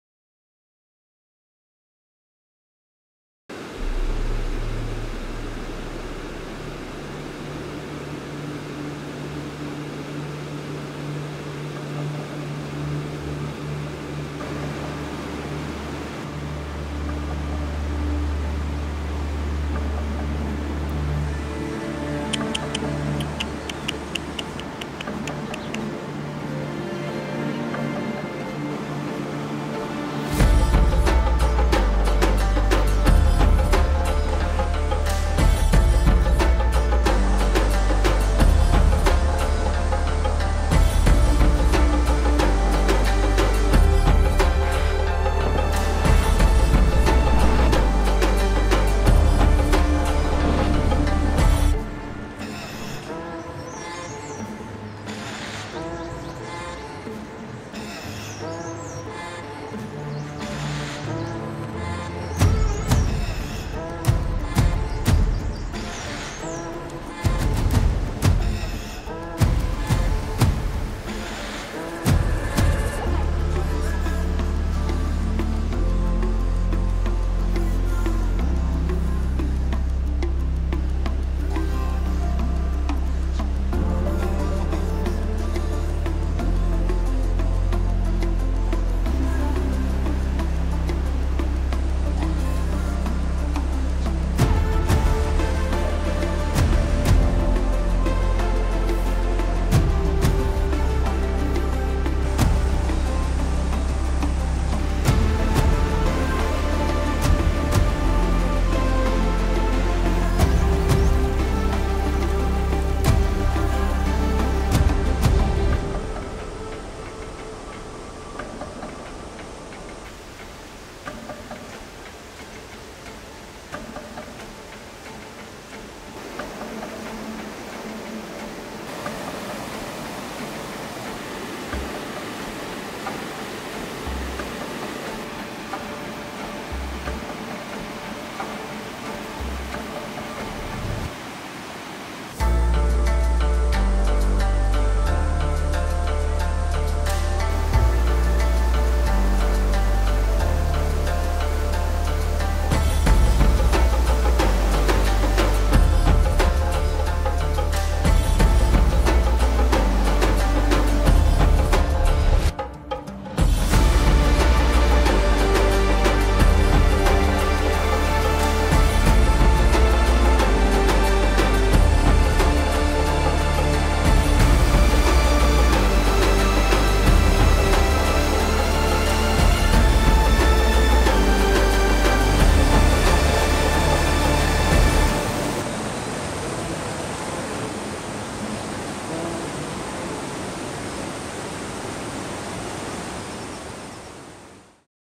NATURE TRAIL | 4K Cinematic Nature Film | Manali Wildlife Sanctuary | Himachal Pradesh | India
A visual journey of Nature in Himalayas through my camera in 4k.

Equipments:
My go to daily camera
Most Used Lens Currently
Portrait & detail (Cheaper alternative) lens
Telephoto Lens
Gimbal I use

A big thank you goes to everyone involved.  You know who you are.